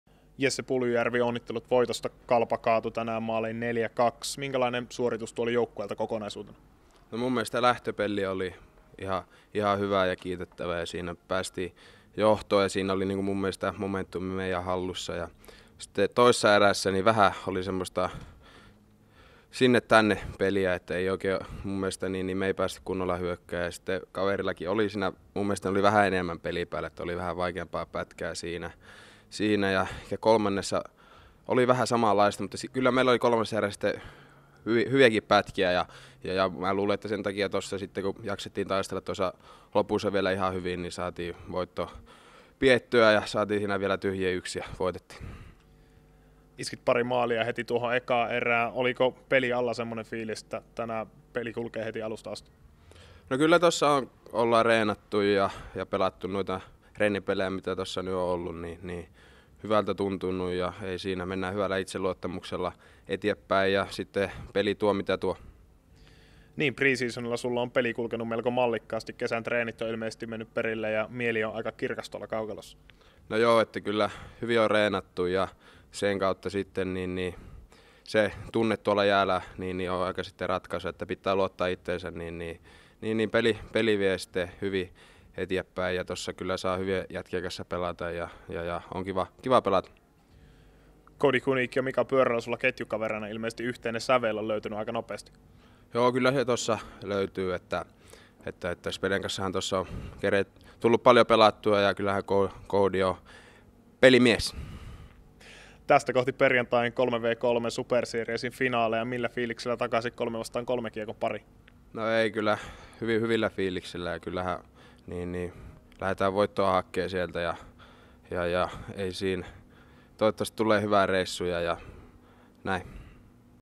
Jesse Puljujärvi: "Hyvällä itseluottamuksella mennään"
Jesse Puljujärven haastattelu Kärppien ja KalPan välisen harjoitusottelun jälkeen.